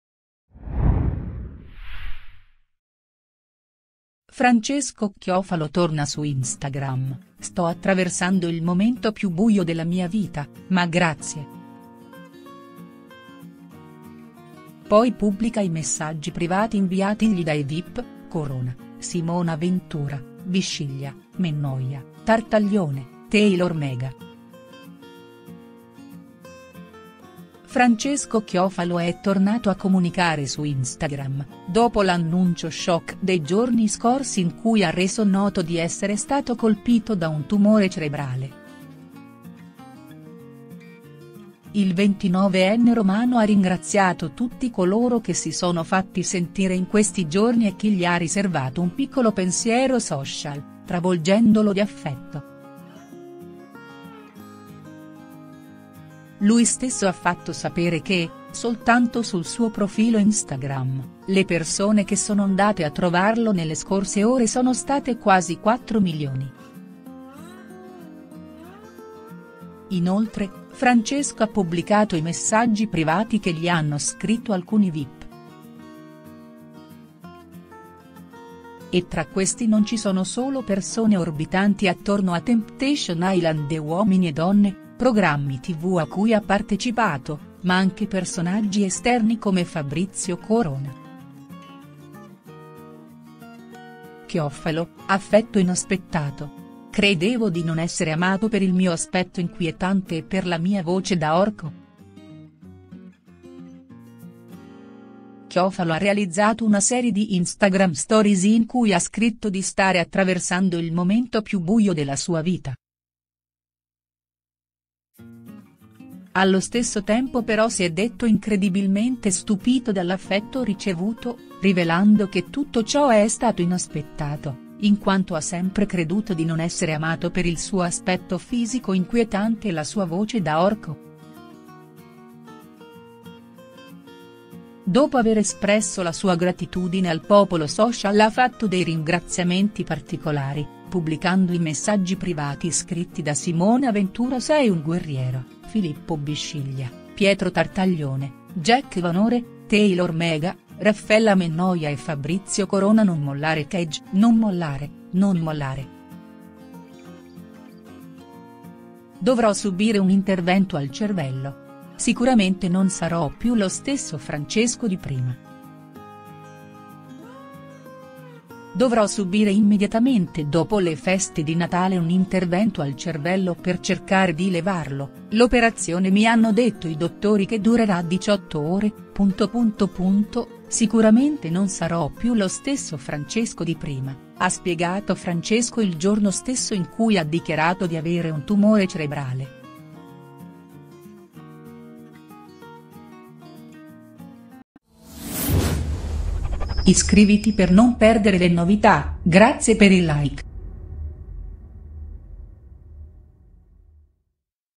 Francesco Chiofalo svela i messaggi privati dei vip: chi gli ha scritto
Francesco Chiofalo torna su Instagram: “Sto attraversando il momento più buio della mia vita, ma grazie”. Poi pubblica i messaggi privati inviatigli dai vip: Corona, Simona Ventura, Bisciglia, Mennoia, Tartaglione, Taylor Mega…
Francesco Chiofalo è tornato a comunicare su Instagram, dopo l’annuncio shock dei giorni scorsi in cui ha reso noto di essere stato colpito da un tumore cerebrale. Il 29enne romano ha ringraziato tutti coloro che si sono fatti sentire in questi giorni e chi gli ha riservato un piccolo pensiero social, travolgendolo di affetto. Lui stesso ha fatto sapere che, soltanto sul suo profilo Instagram, le persone che sono ‘andate a trovarlo’ nelle scorse ore sono state quasi 4 milioni. Inoltre, Francesco ha pubblicato i messaggi privati che gli hanno scritto alcuni vip. E tra questi non ci sono solo persone orbitanti attorno a Temptation Island e Uomini e Donne (programmi tv a cui ha partecipato), ma anche personaggi esterni come Fabrizio Corona.
Chiofalo: “Affetto inaspettato. Credevo di non essere amato per il mio aspetto inquietante e per la mia voce da orco”
Chiofalo ha realizzato una serie di Instagram Stories in cui ha scritto di stare “attraversando il momento più buio della sua vita”. Allo stesso tempo però si è detto incredibilmente stupito dall’affetto ricevuto, rivelando che tutto ciò è stato “inaspettato“, in quanto ha sempre creduto di non essere amato per il suo “aspetto fisico inquietante e la sua voce da orco.” Dopo aver espresso la sua gratitudine al popolo social ha fatto dei “ringraziamenti particolari“, pubblicando i messaggi privati scritti da Simona Ventura (“Sei un guerriero”), Filippo Bisciglia, Pietro Tartaglione, Jack Vanore, Taylor Mega, Raffella Mennoia e Fabrizio Corona (“Non mollare caz… non mollare, non mollare”).
“Dovrò subire un intervento al cervello. Sicuramente non sarò più lo stesso Francesco di prima”
“Dovrò subire immediatamente dopo le feste di Natale un intervento al cervello per cercare di levarlo: l’operazione mi hanno detto i dottori che durerà 18 ore […] Sicuramente non sarò più lo stesso Francesco di prima”, ha spiegato Francesco il giorno stesso in cui ha dichiarato di avere un tumore cerebrale.